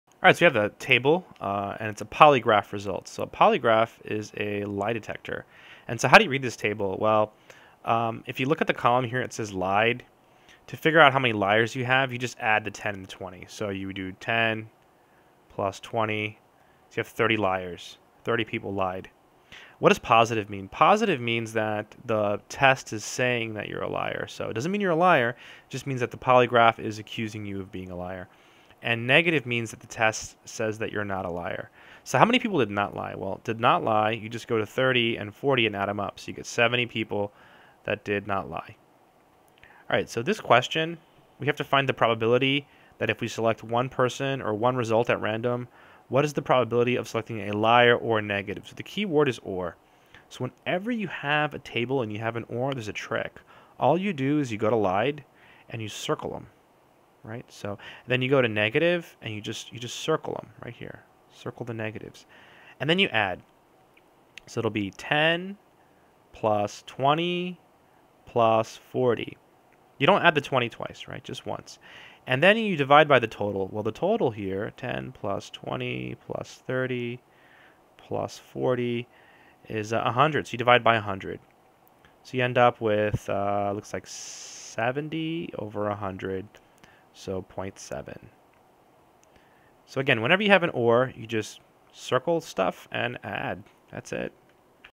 How to Compute a Probability from a Table using the Addition Rule Example 1
How to Compute a Probability from a Table Addition Rule Example 1. Just a simple example of how to use a table and the addition rule to compute a probability. In this example we look at the results of a Polygraph Test.